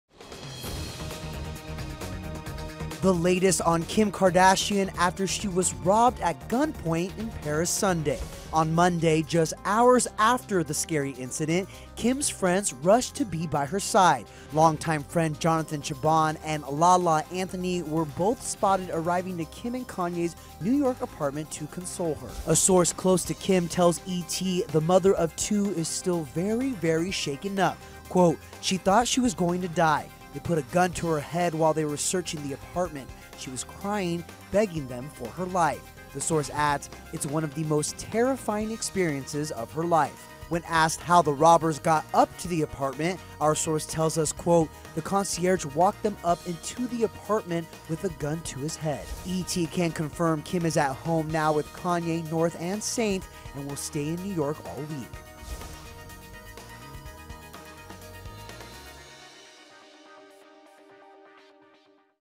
EXCLUSIVE: Kim Kardashian Still 'Shaken Up' After Armed Robbery: 'She Thought She Was Going to Di…
A source close to Kim tells ET how robbers got into her Paris apartment.